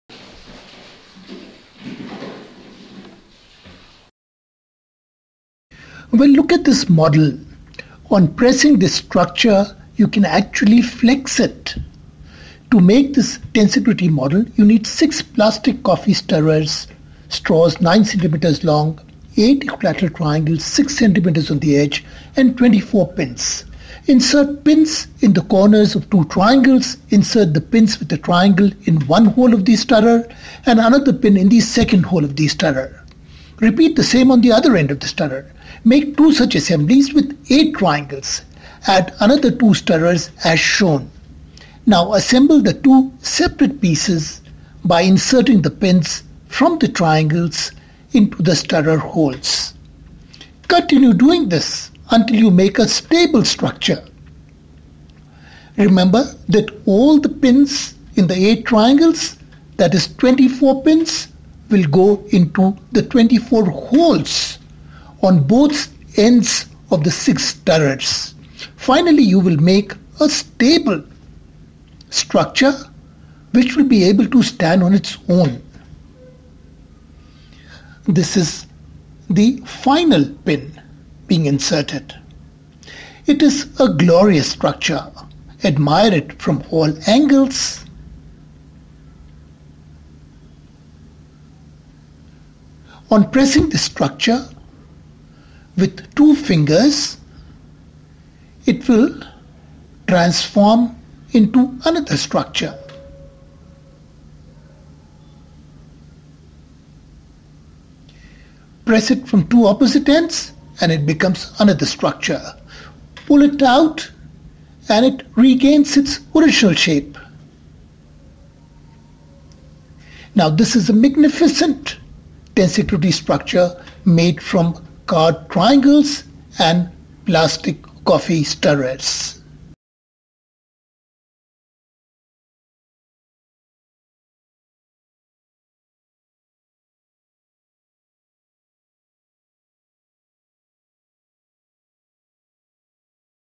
Tensegrity with Triangles and sticks | English
Look at this model. On pressing this structure you can flex it.  To make this you need 6 plastic coffee stirrer straws 9-cm long, 8 equilateral triangles 6-cm edge and 24 pins. Insert pins in the corners of two triangles. Insert the pin with a triangle in one hole of the stirrer and another pin in the second hole of the stirrer. Repeat the same on the other end of the stirrer. Make two such assemblies with 8 triangles. Add another two stirrers as shown. Now assemble the two separate pieces by inserting the pins from the triangles into the stirrer holes. Continue doing this until you make a stable structure. Remember that all the pins in the 8 triangles i.e. 24 pins will go into 24 holes of the 6 stirrers. Finally you will make a stable structure which will be able to stand on its own. It is a glorious structure. Admire it from all angles. On pressing the structure with two fingers it will transform into another structure. On pulling it with two fingers it will regain its original form.